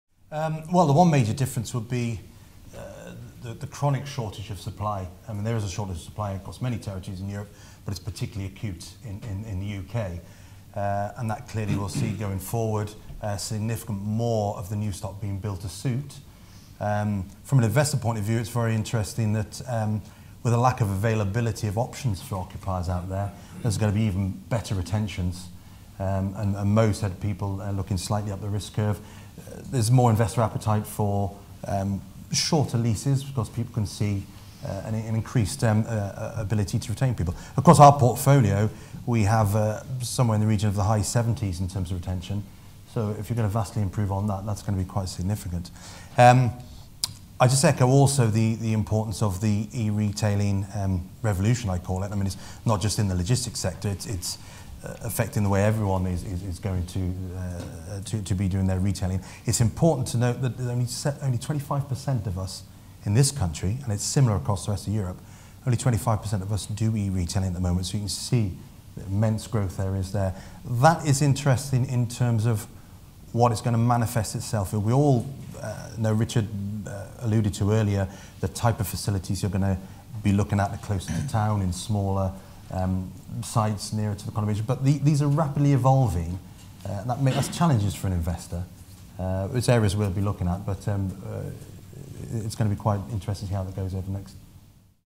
Logistics - Trends, UK vs Europe
Mike Forster, Managing Director UK & Ireland of PointPark Properties (P3) highlights some of the key differences between the markets in Europe in terms of the impact of market trends including e-commerce.

© PropertyEU European Logistics Investment Briefing, London, January 2014